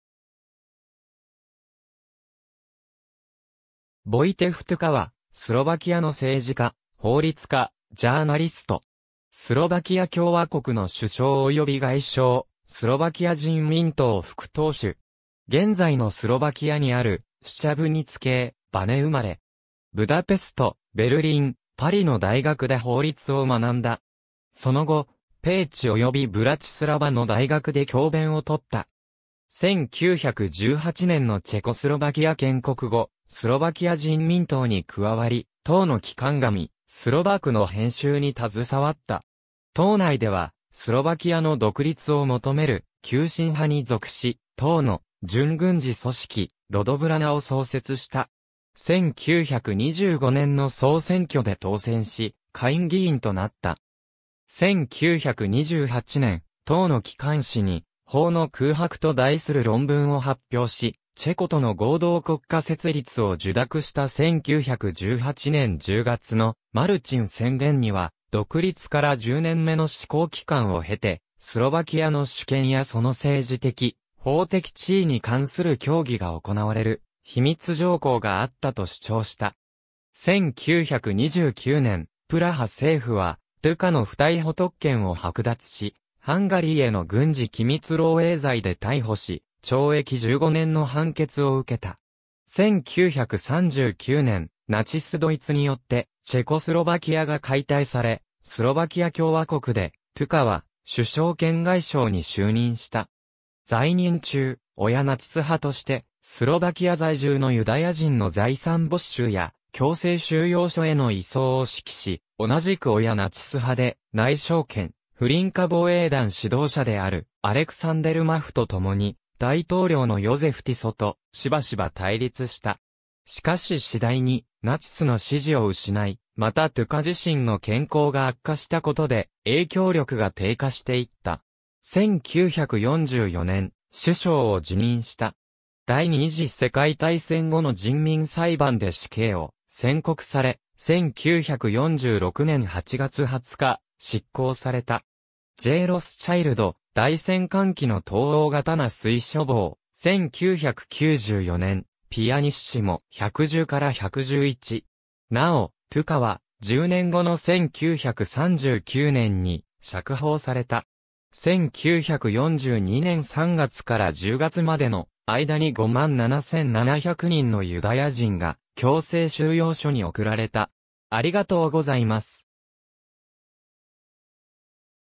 ヴォイテフ・トゥカ
オーストリア＝ハンガリー帝国のスロバキア人
ヴォイテフ・トゥカ（スロバキア語: Vojtech "Béla" Tuka、1880年7月4日 - 1946年8月20日）は、スロバキアの政治家、法律家、ジャーナリスト。
スロバキア共和国の首相および外相、スロバキア人民党副党首。
現在のスロバキアにあるシュチャヴニツケー・バネ生まれ。
ブダペスト、ベルリン、パリの大学で法律を学んだ。
その後、ペーチおよびブラチスラヴァの大学で教鞭をとった。
1918年のチェコスロバキア建国後、スロバキア人民党に加わり、党の機関紙『スロバーク』の編集に携わった。
党内ではスロバキアの独立を求める急進派に属し、党の準軍事組織ロドブラナ（祖国防衛軍）を創設した。
1925年の総選挙で当選し下院議員となった。
1928年、党の機関紙に「法の空白」と題する論文を発表し、チェコとの合同国家設立を受諾した1918年10月のマルチン宣言には、独立から10年目の試行期間を経て、スロバキアの主権やその政治的・法的地位に関する協議が行われる秘密条項があったと主張した。
1929年、プラハ政府はトゥカの不逮捕特権を剥奪し、ハンガリーへの軍事機密漏洩罪で逮捕し、懲役15年の判決を受けた。
1939年、ナチス・ドイツによってチェコスロバキアが解体され、スロバキア共和国で、トゥカは首相兼外相に就任した。
在任中、親ナチス派として、スロバキア在住のユダヤ人の財産没収や強制収容所への移送を指揮し、同じく親ナチス派で内相兼フリンカ防衛団指導者であるアレクサンデル・マフとともに大統領のヨゼフ・ティソとしばしば対立した。
しかし次第にナチスの支持を失い、またトゥカ自身の健康が悪化したことで、影響力が低下していった。
1944年、首相を辞任した。
第二次世界大戦後の人民裁判で死刑を宣告され、1946年8月20日執行された。
^ J・ロスチャイルド『大戦間期の東欧』刀水書房、1994年、pp.110-111。
なお、トゥカは、10年後の1939年に釈放された。
^ 1942年3月から10月までの間に57700人のユダヤ人が強制収容所に送られた。